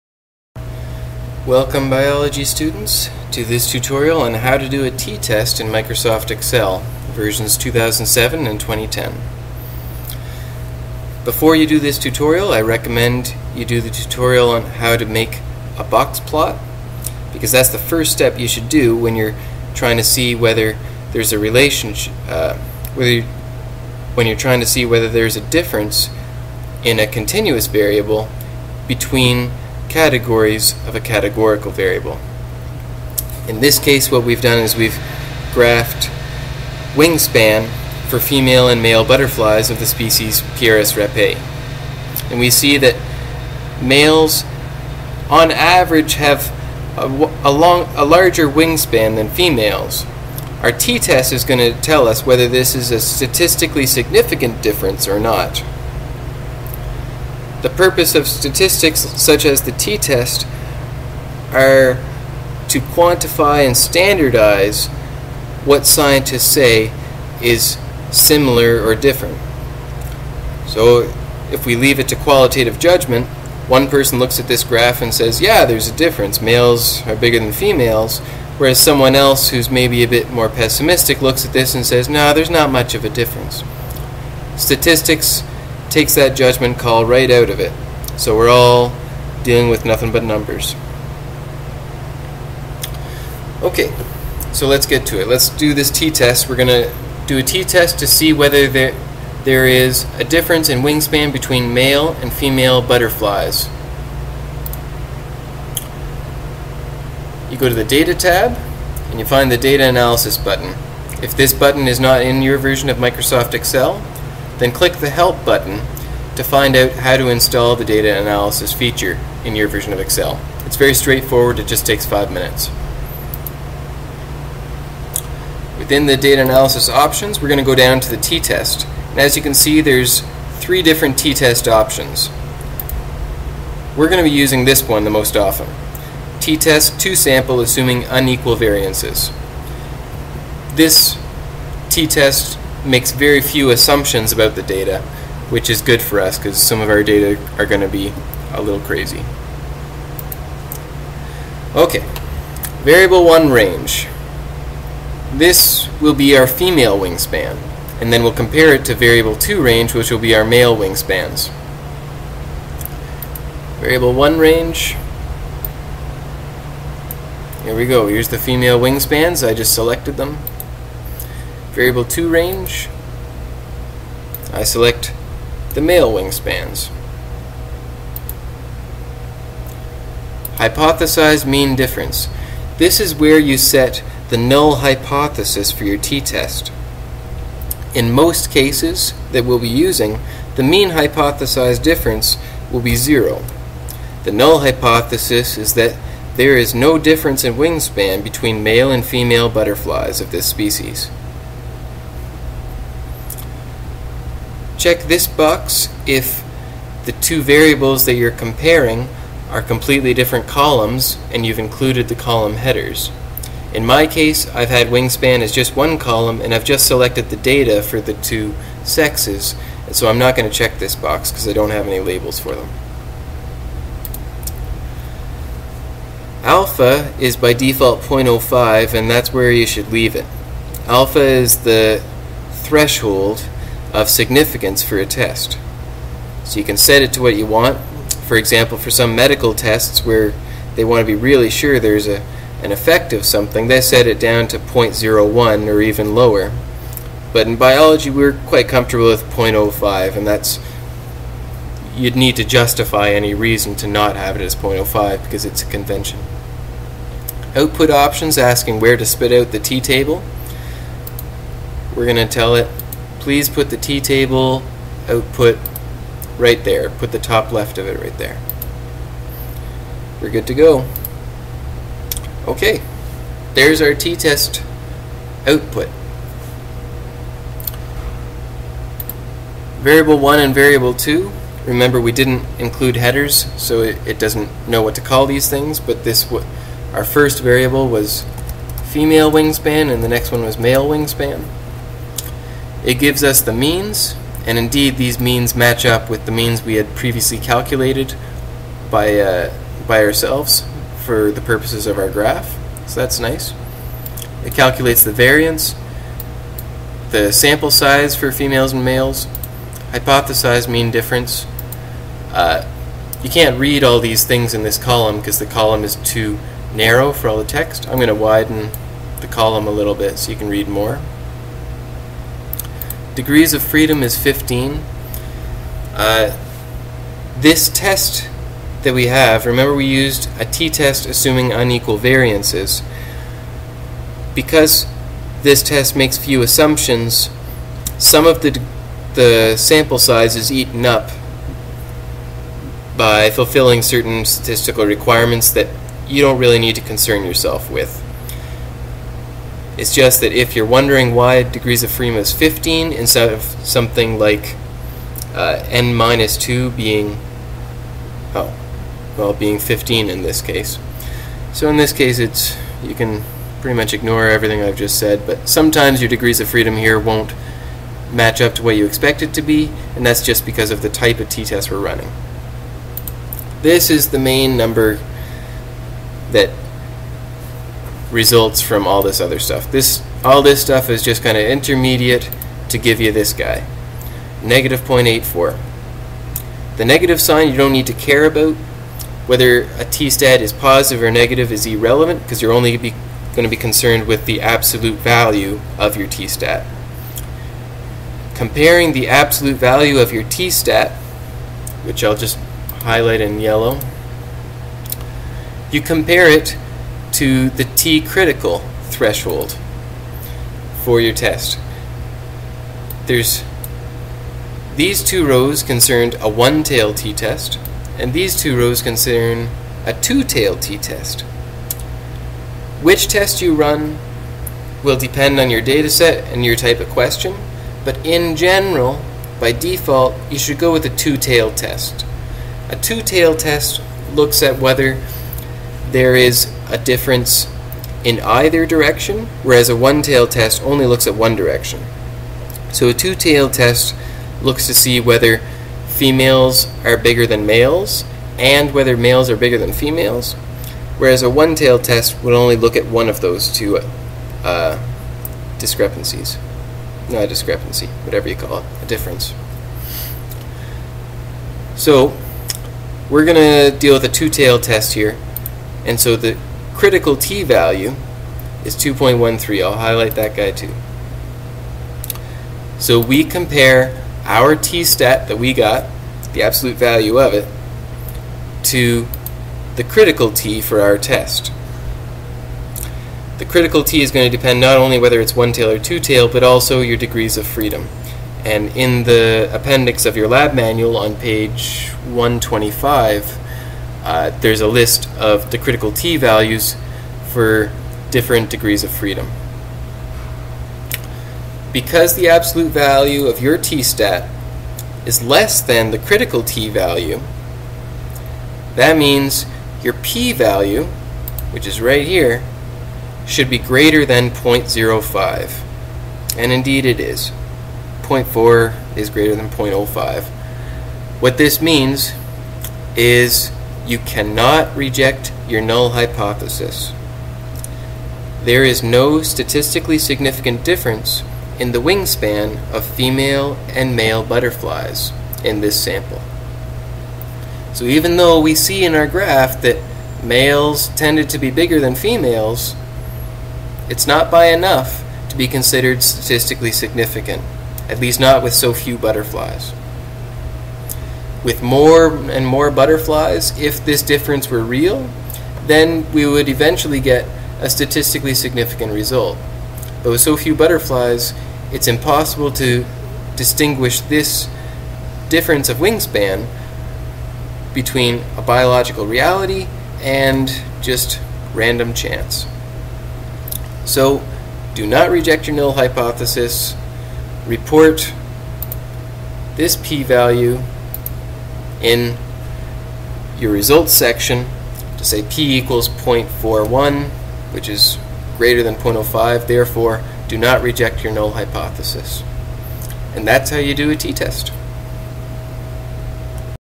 5) How to do a t-test in Excel - for Carleton University BIOL 1004 & 1104
How to run a t-test in Microsoft Excel 2007/2010.  One of several video tutorials created for students in Carleton University's Foundations of Biology II courses (BIOL 1004 & BIOL 1104).  Created by Lab Coordinator Jay Fitzsimmons in January 2012.